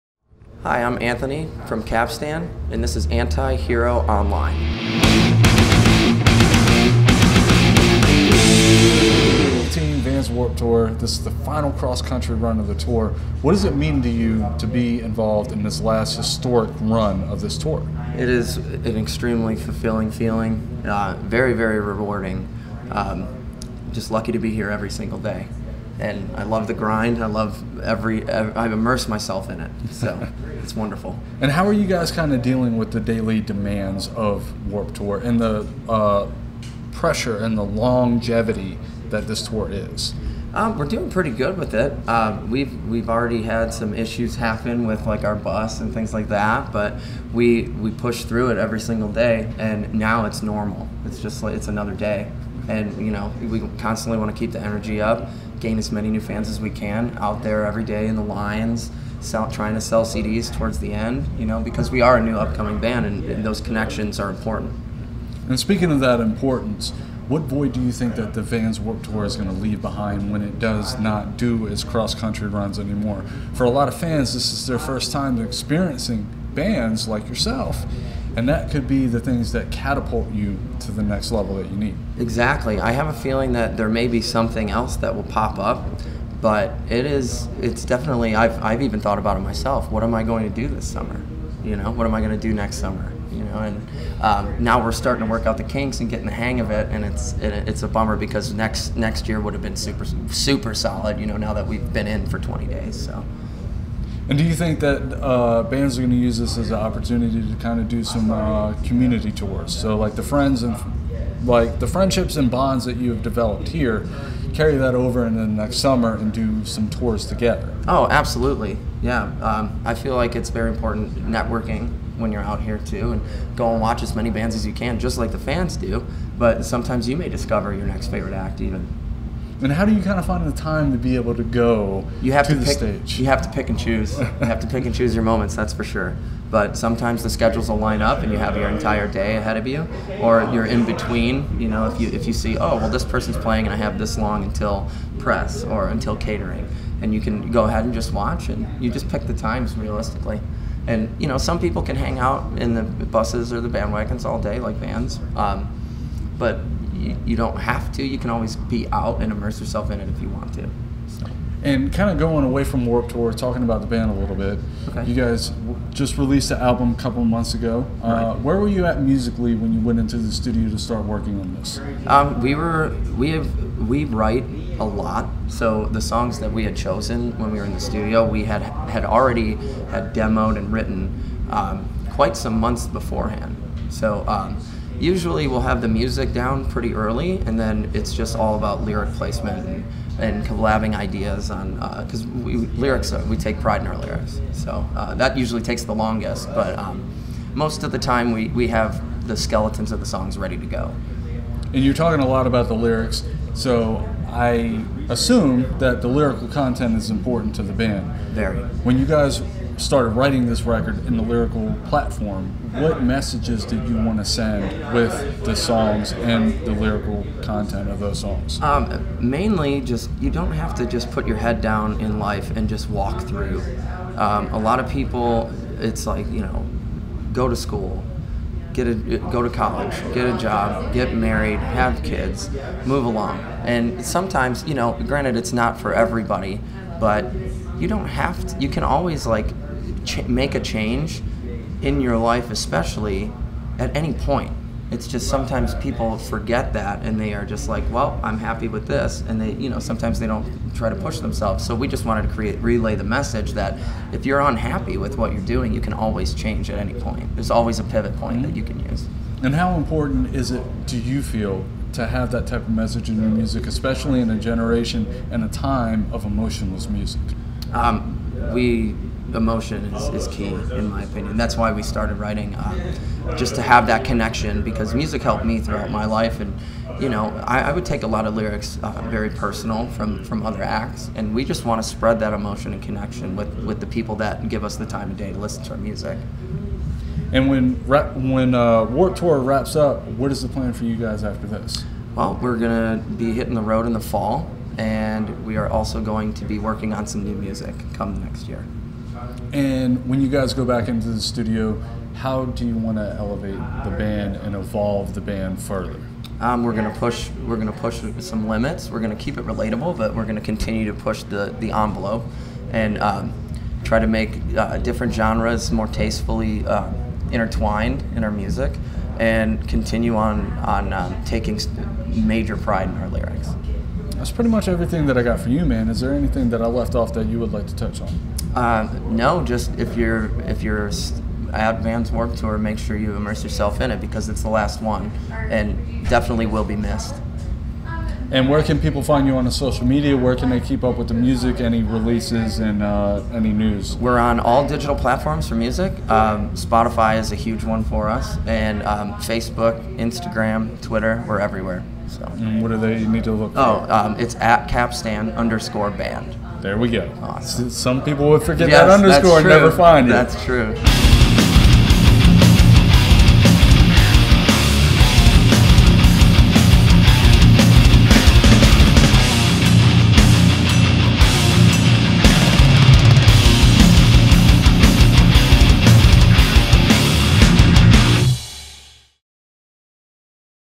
Interview with Anthony DeMario of Capstan at Vans Warped Tour 2018
Antihero Magazine had the chance to catch up with many of the bands on the 2018 run of the Van’s Warped Tour when it stopped in Charlotte NC at the PNC Music Pavilion. During the conversations, the bands touched on the demands of the tour, staying healthy on the road, the void Warped Tour is going to leave on the summers going forward, and new music from some of the bands.